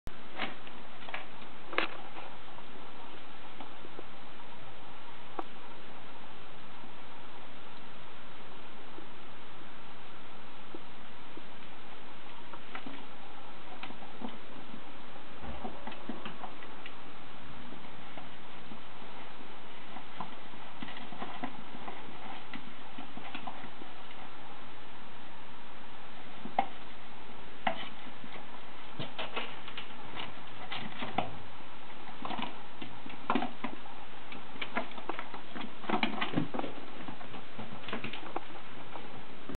Ferrety moves his toy up to stairs
Ferrety's